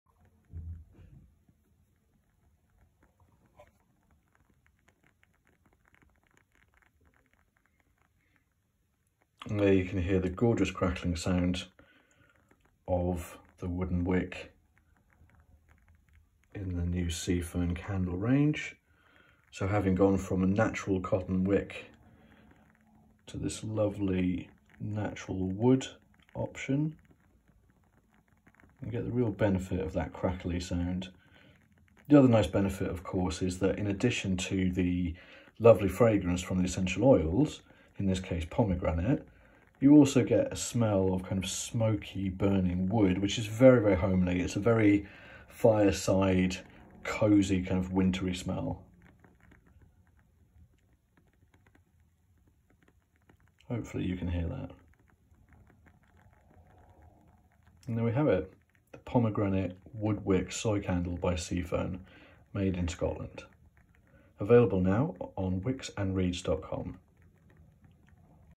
Wicks & Reeds Candle Review - Woodwick Pomegranate Candle, Handmade in Scotland
Check out the amazing crackle sound on this woodwick candle!
Part of our longer candle review video of the new range of Sea Fern Woodwick Candles made in Scotland and available